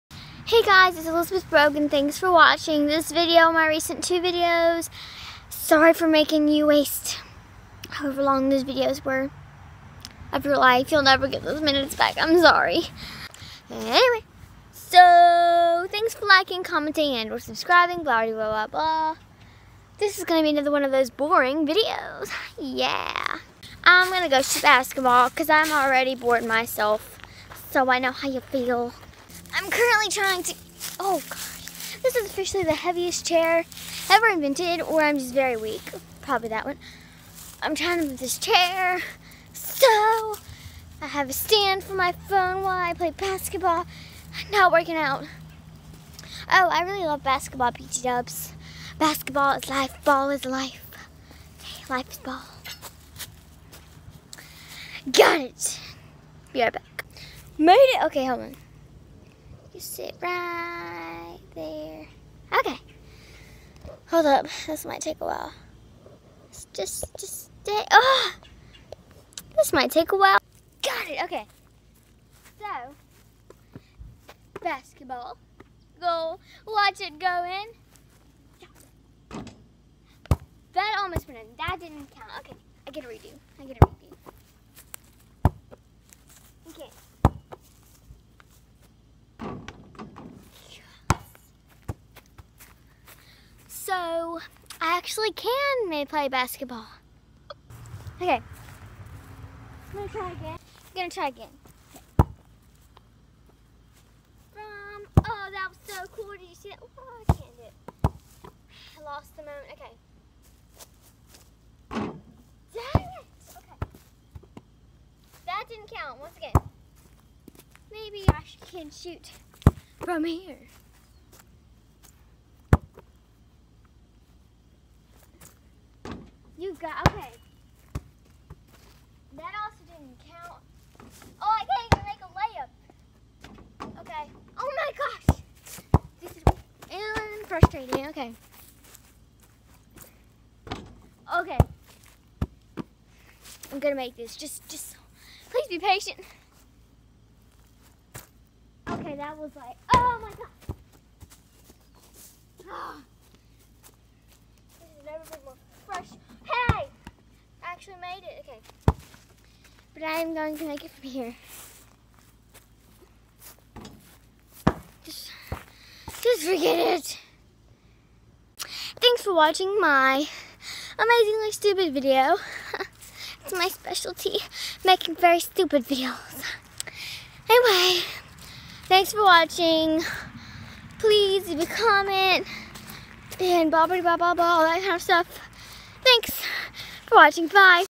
My lammeeeoo basketball skillzz
In this video you will see my good/bad (more on the bad side) skillzz for basketball. WARNING: BORING ALERT😊❤️